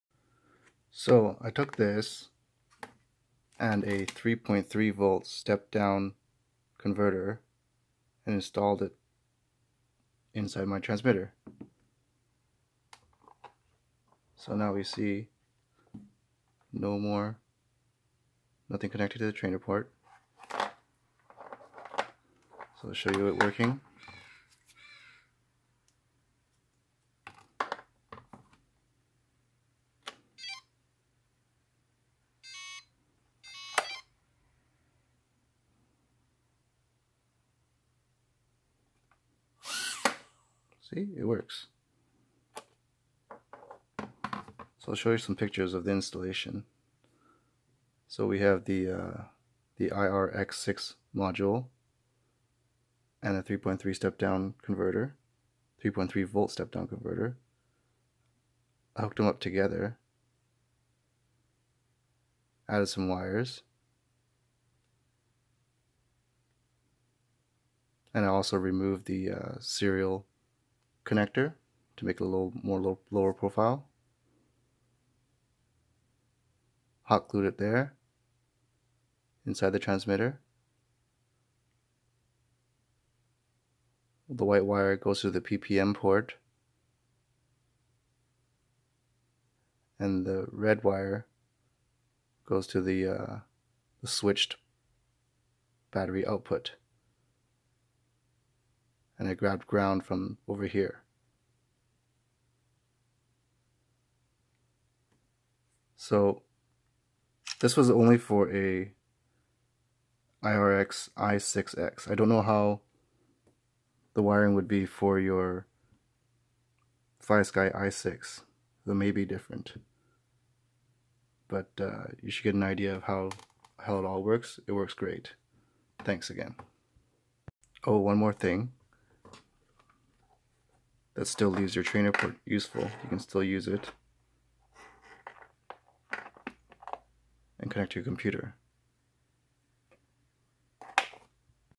iRangeX iRX6 multiprotocol module mod
This mod allows a much easier and cleaner operation of the irx6. It also leaves the trainer port open for simulators.

-this one may be better because it has a higher input voltage range but costs more